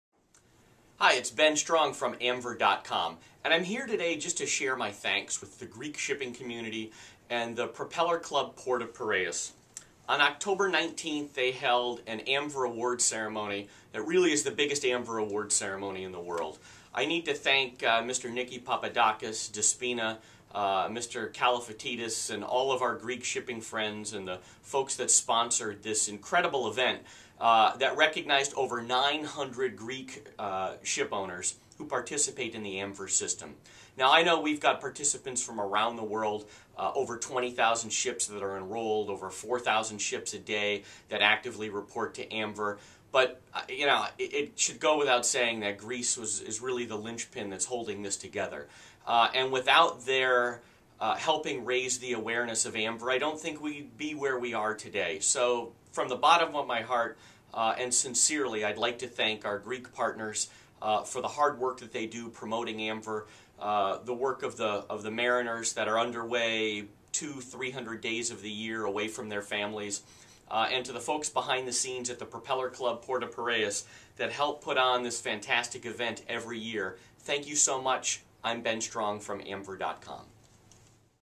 2010 Greek Amver Awards
Amver is the Automated Mutual Assistance Vessel Rescue System, an international search and rescue system, sponsored by the United States Coast Guard that uses commercial ships to rescue people at sea.

The Propeller Club Port of Piraeus held their Amver Awards on October 19, 2010 in Athens, Greece at the Intercontinental Hotel. Over 900 ships earned awards and were recognized for participating in the Amver safety network.